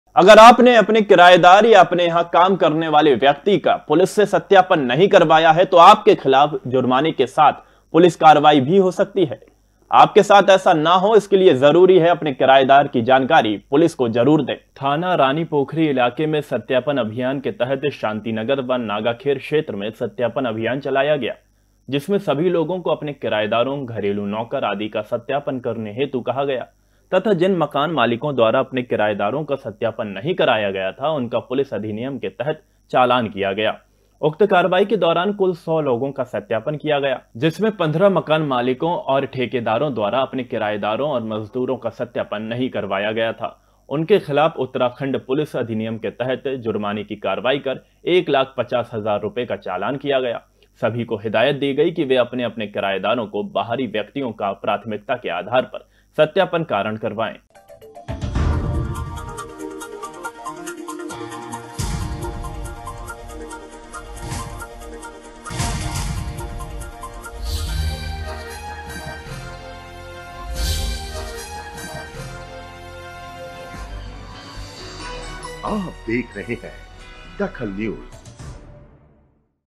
DAKHAL NEWS ठेकेदारों और माकन मालिकों पर नकेल
ठेकेदारों और माकन मालिकों पर नकेल
किरायदार की जानकरी नहीं देने पर जुर्माना